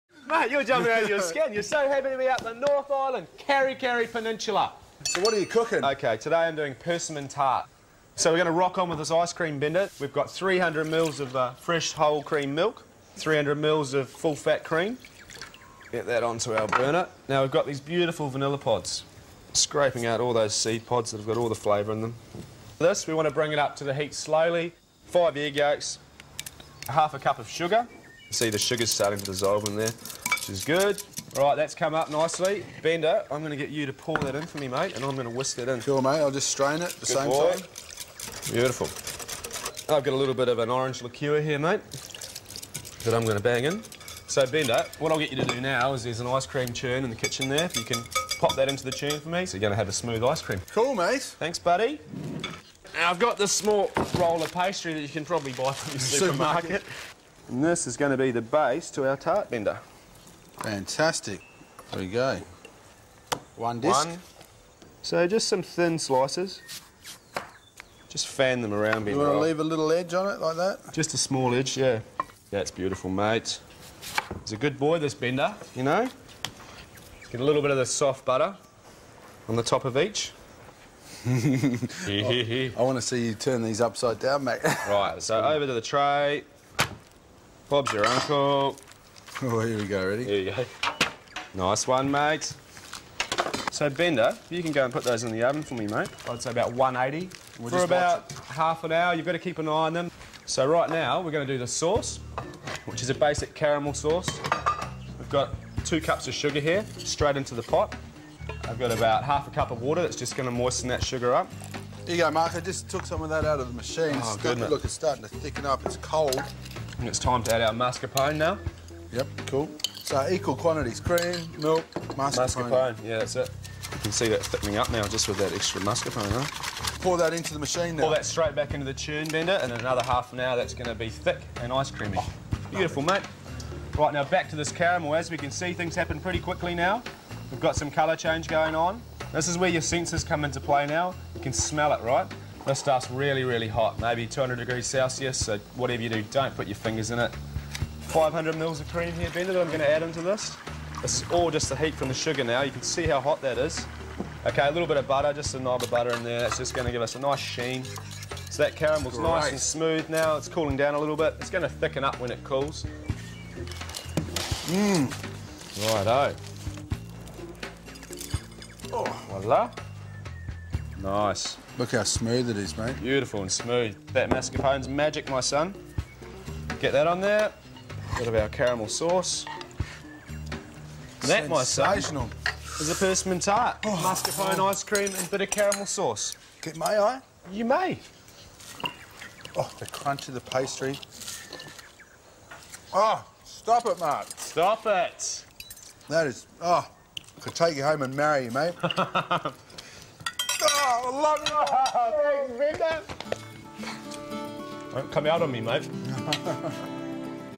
Mark Gardner chef. Persimmon tart
Mark cooking persimmon tart with mascapone vanilla icecream & caramel sauce in Northland NZ.